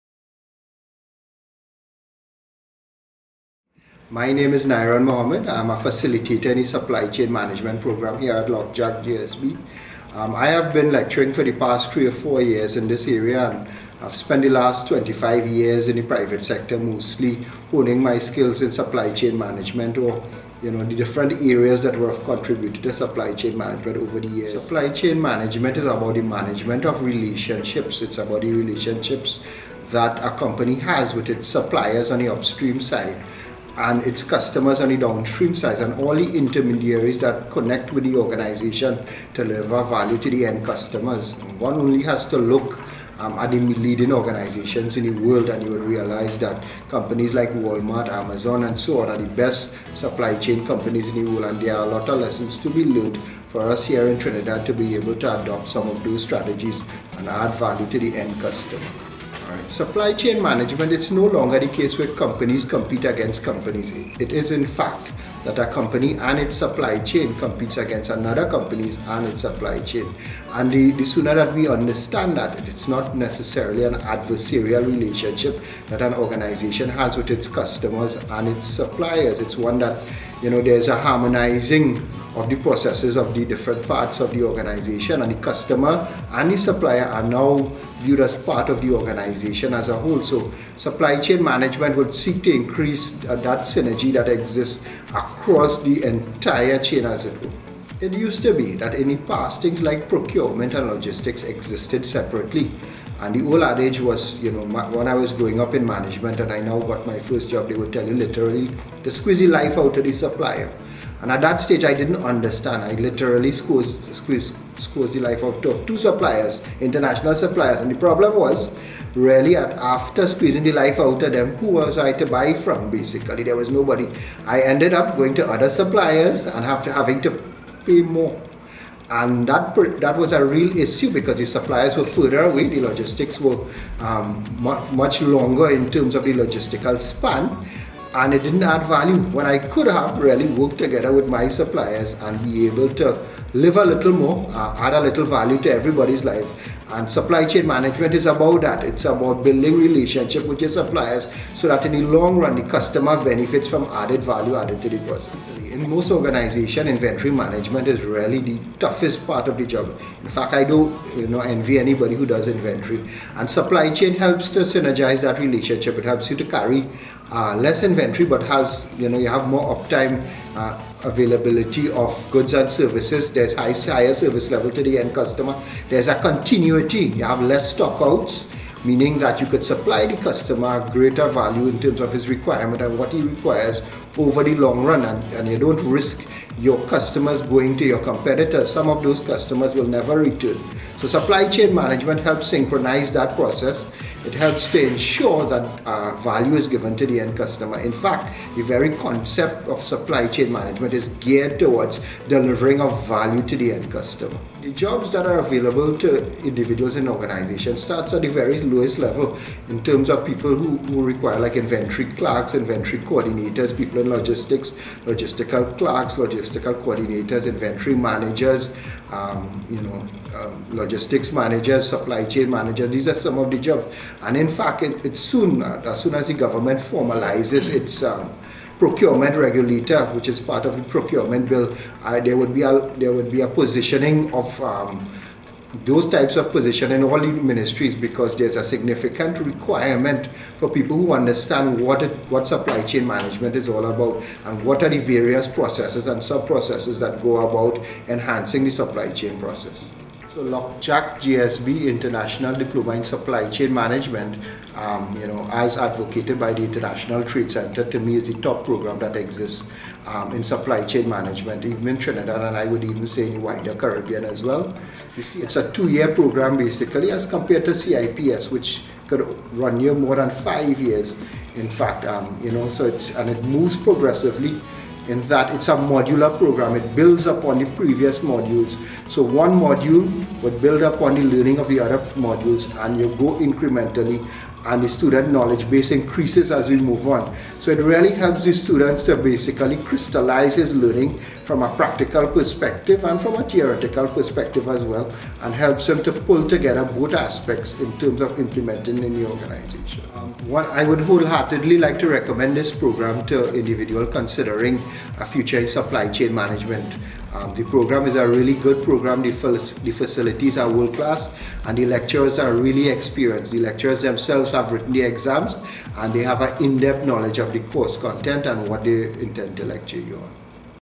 Faculty Nyron Mohammed tells of the Modular Learning System in Supply Chain Management
Nyron Mohammed shares information on International Trade Centre (ITC), Geneva Switzerland program on Modular Learning System (MLS) in Supply Chain Management (SCMè).
The UWI-Lok Jack GSB is licensed by ITC to offer this programme in Trinidad & Tobago. Through its 18 modules, the MLS-SCMè covers all stages in the purchasing and supply management process.
The MLS-SCMè is a practical, continuously updated and supported training pack that can help you make effective international purchasing and supply chain management become a key factor for your company's competitiveness and success.

The expected impact of the program can be measured in terms of increased effectiveness and efficiency of your the total supply chain operations. Tangible results will include:

·        Reduced lead times and improved response time to market changes,
·        Promotion of supply innovation and reduction of inventories,
·        Use of optimum quality materials,
·        Ensuring continuity of production supplies, and
·        Reduction in the cost of purchased inputs.

This programme is targeted at procurement/purchasing officers, buyers, managers and all those personnel who cover functional aspects in the supply chain process. These courses are based on an interactive and participative approach, which support group training as well as individually based open learning.